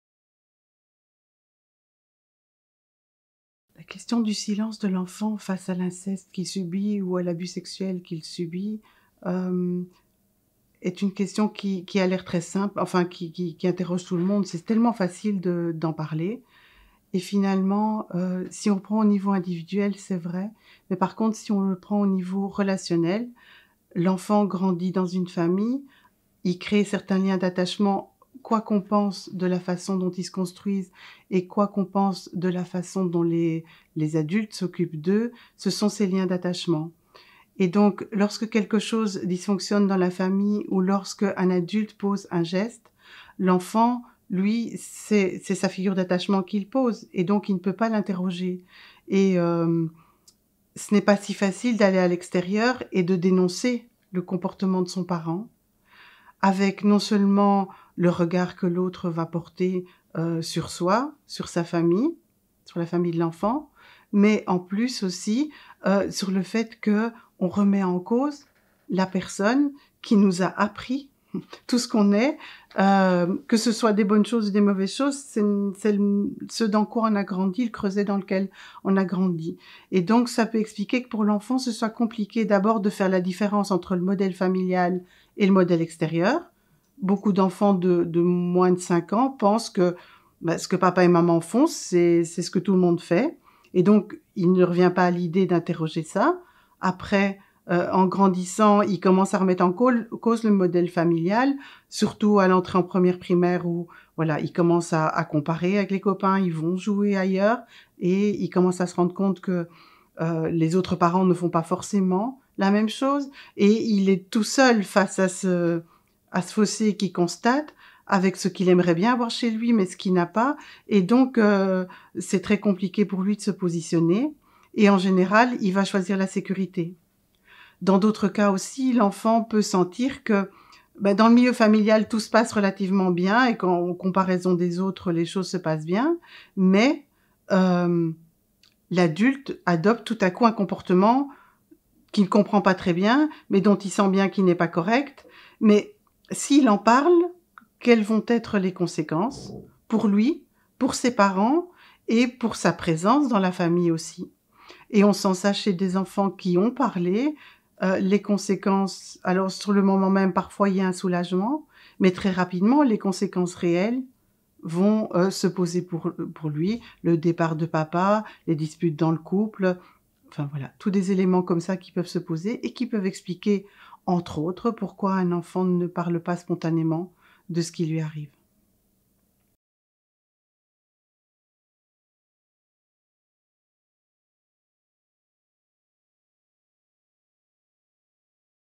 Comment comprendre qu’un enfant ne parle pas de l’inceste qu’il subit ?
Un entretien avec Françoise Hoornaert (03:09), psychologue clinicienne, coordinatrice de l’équipe SOS Parents-Enfants de Tournai